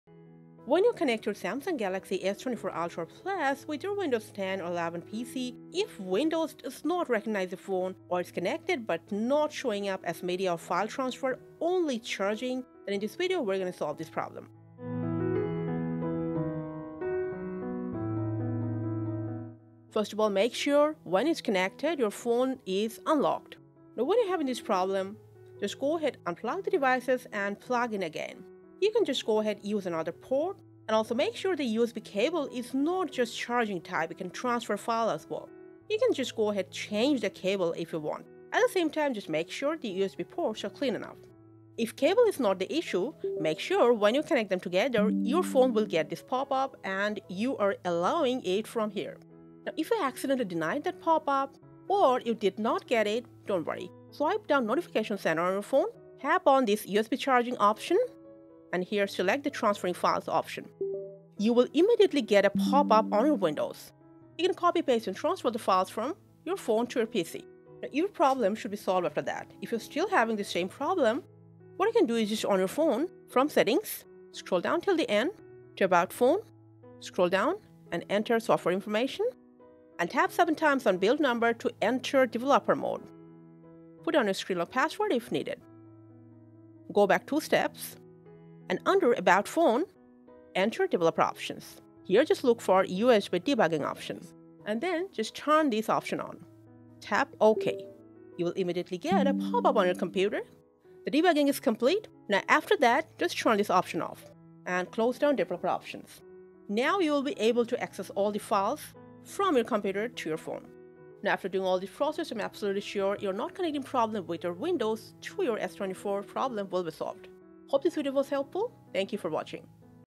Galaxy S24 Ultra/Plus: Not Connecting to Windows PC? - Fixed Samsung!
Samsung Galaxy S24/S24+/S24 won’t connect to your Windows Computer and won’t show the connection pop-up? Windows PC does not recognise your Samsung Galaxy S24 series Phone when try to transfer files. And only charges the phone? Worry not, the Fix369 team has a video for you.

In this video, we will show you 3 simple yet effective solutions to fix Windows PCs not connecting or recognizing Samsung Galaxy S24 Ultra or Plus effectively, so you can easily share files between them.

0:00 Samsung Galaxy S24 Ultra/Plus not connecting to Windows PC.
0:19 Solution 1: Basic Troubleshooting
0:41 Solution 2: Allow Permissions
1:09 Solution 3: USB Debugging